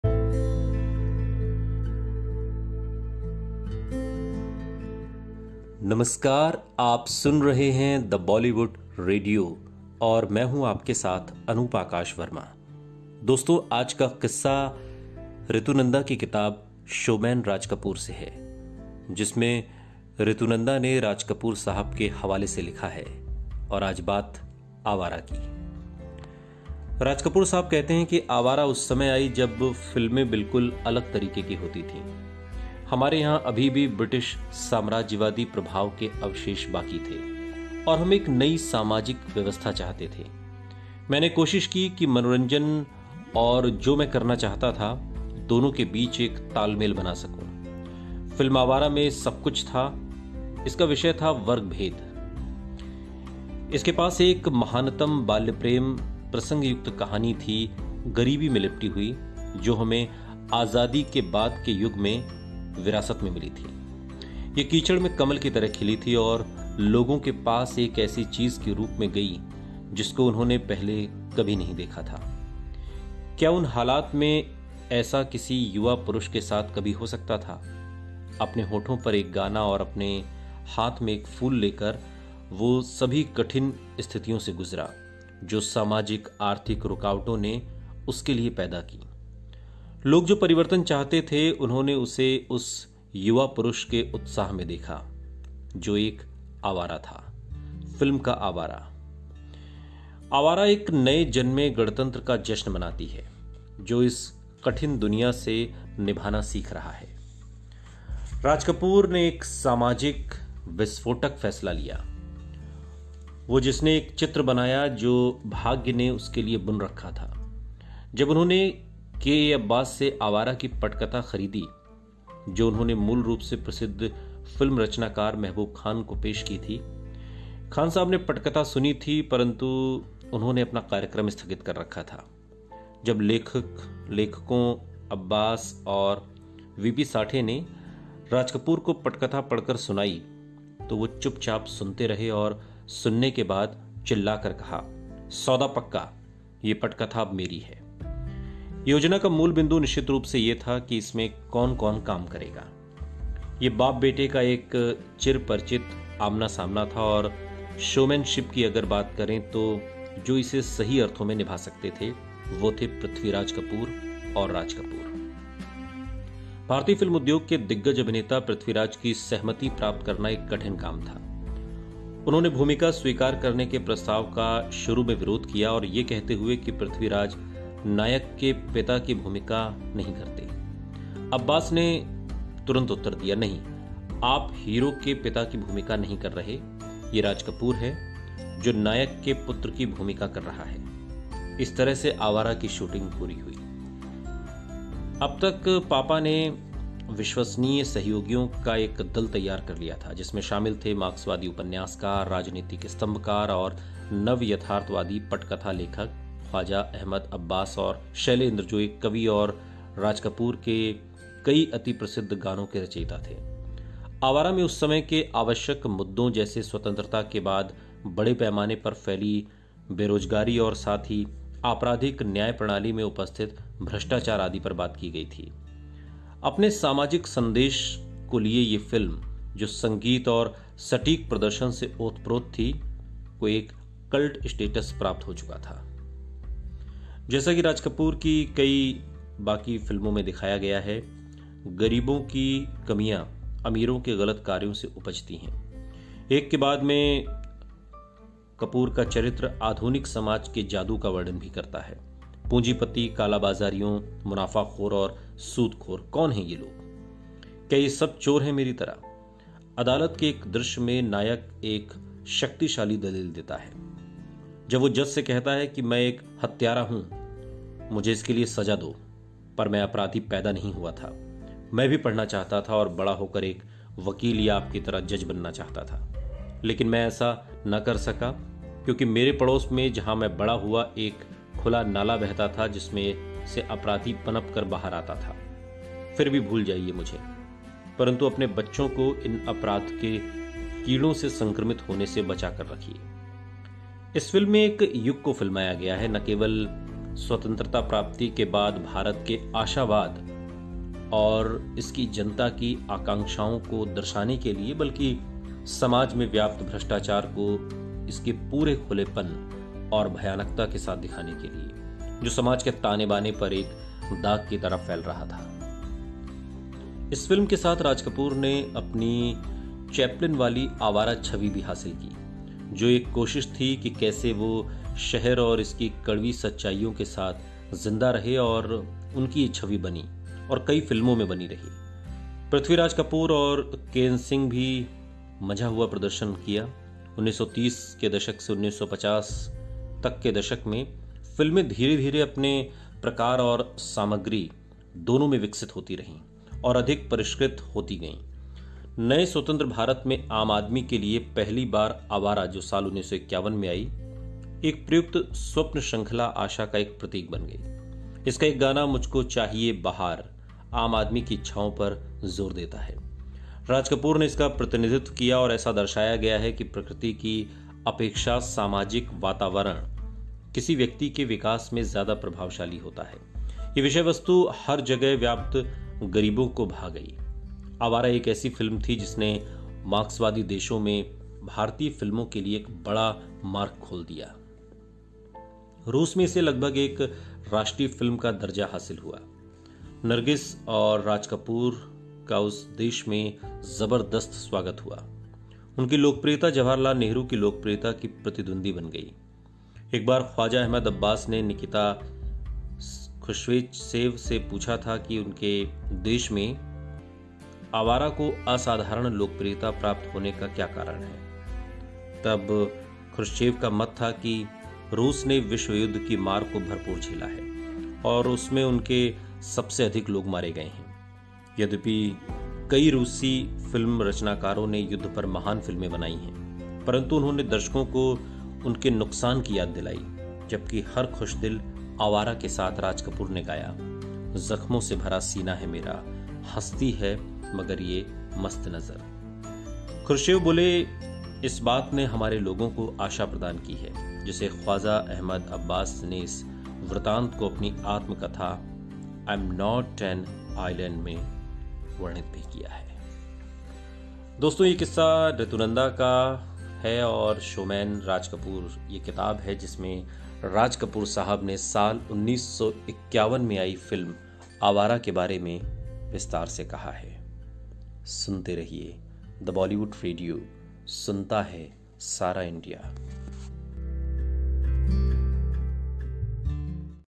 Raj Kapoor की फिल्म आवारा की कहानी, जिसने Nehru की लोकप्रियता को दी थी चुनौती
आज़ादी के बाद बनी राज कपूर की आवारा कई मायनों में हिन्दुस्तान के मौजूदा हालात को बयां करती है, इस फिल्म ने राज कपूर की लोकप्रियता को आसमान पर पहुंचा दिया था. उस ज़माने में राज कपूर की लोकप्रियता की तुलना तत्कालीन प्रधानमंत्री पंडित जवाहर लाल नेहरू से होने लगी थी। सुनिए पूरी कहानी का PODCAST अनूप आकाश वर्मा (Anoop Aakash Verma) के साथ. @thebollywoodradio